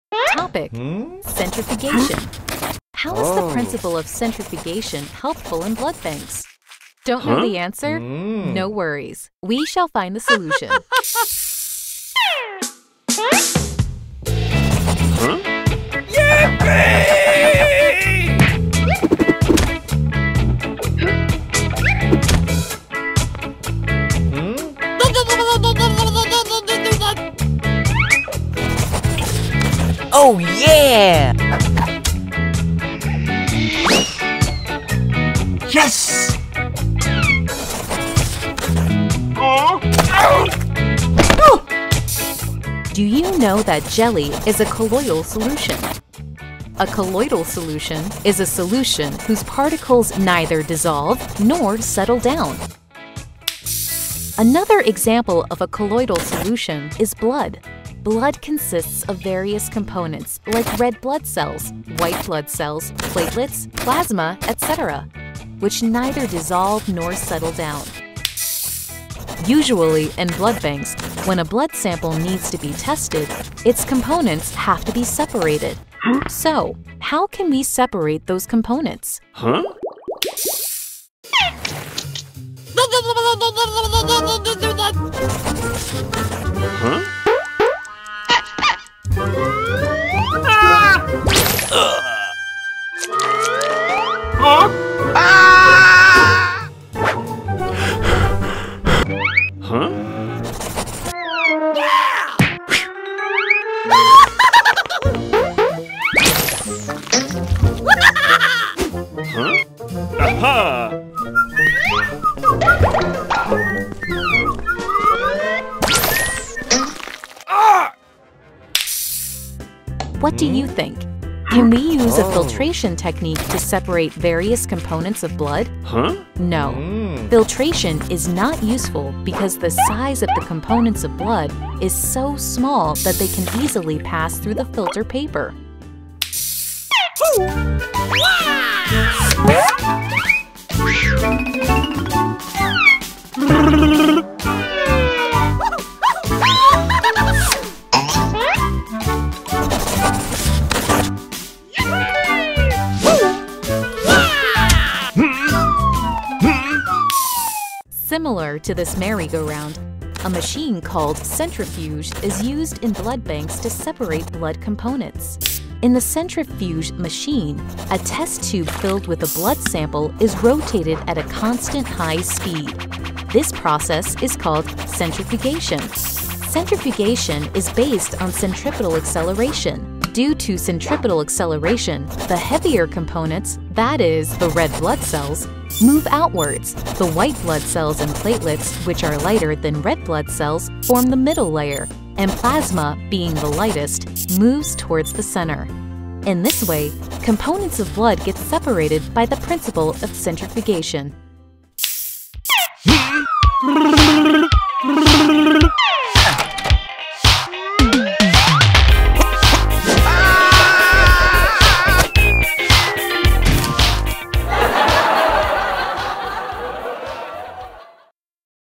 How is the principle of centrifugation helpful in blood banks?
Topic: Centrifugation
Do you know that jelly is a colloidal solution?
A colloidal solution is a solution whose particles neither dissolve nor settle down.
Another example of a colloidal solution is blood.
Blood consists of various components like red blood cells, white blood cells, platelets, plasma, etc., which neither dissolve nor settle down.
Usually, in blood banks, when a blood sample needs to be tested, its components have to be separated.
So, how can we separate those components?
Can we use a filtration technique to separate various components of blood?
No, filtration is not useful because the size of the components of blood is so small that they can easily pass through the filter paper.
Similar to this merry go round, a machine called centrifuge is used in blood banks to separate blood components.
In the centrifuge machine, a test tube filled with a blood sample is rotated at a constant high speed.
This process is called centrifugation.
Centrifugation is based on centripetal acceleration.
Due to centripetal acceleration, the heavier components, that is, the red blood cells move outwards, the white blood cells and platelets which are lighter than red blood cells form the middle layer, and plasma being the lightest moves towards the center.
In this way, components of blood get separated by the principle of centrifugation.